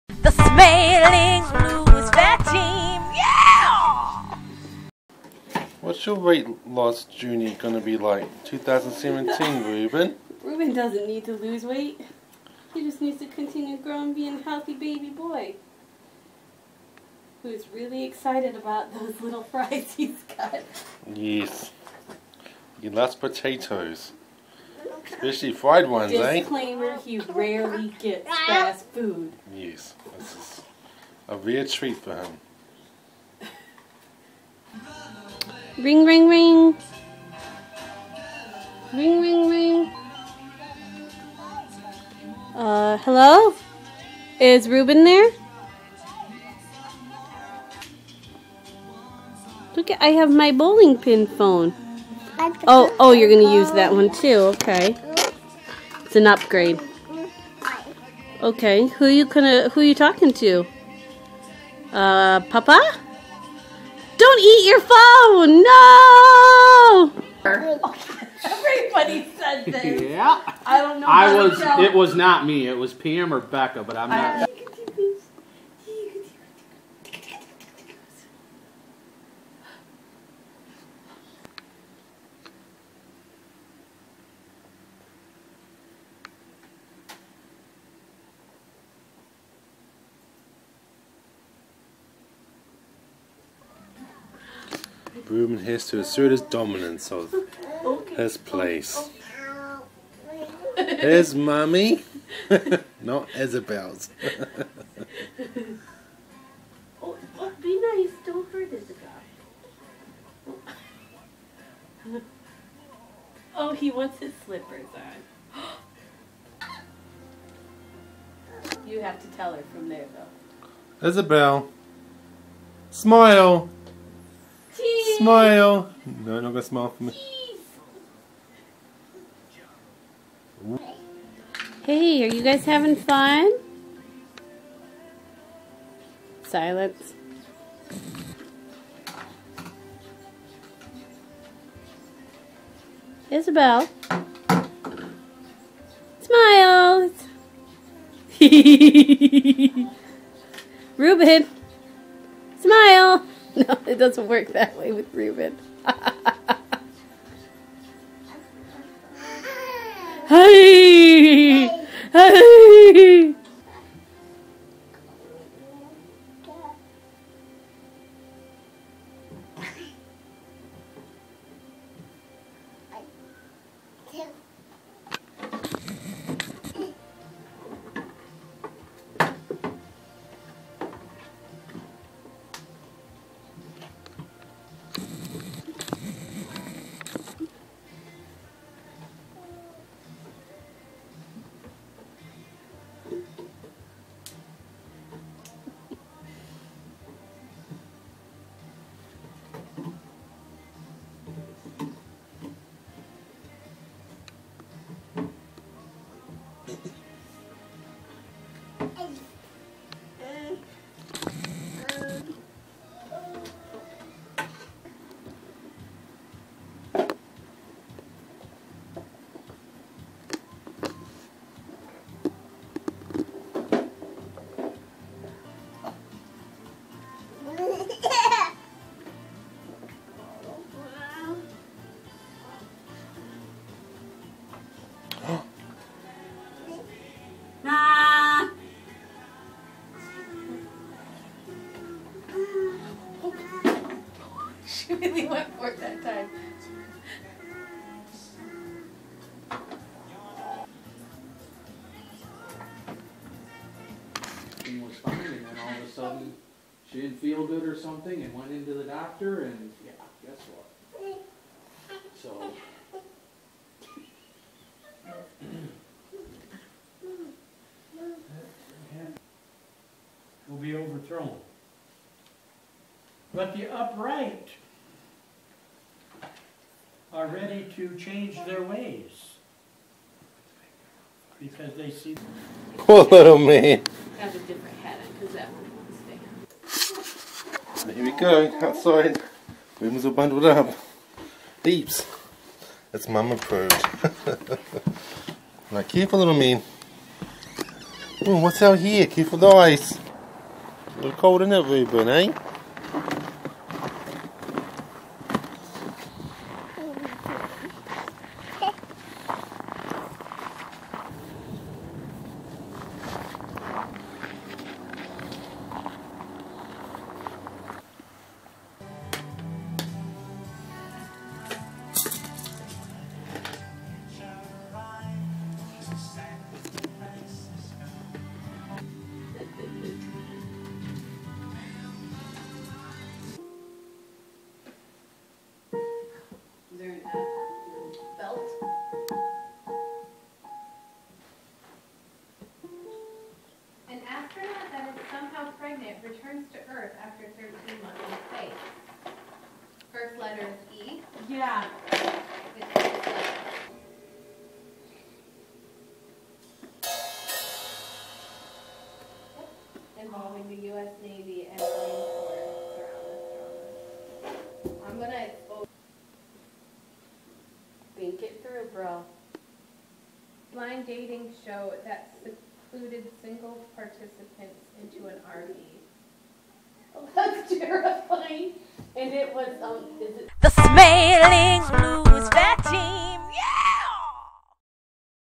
Journal: January 2017 #1
Video clips of mostly Reuben from 1st till 8th January 2017.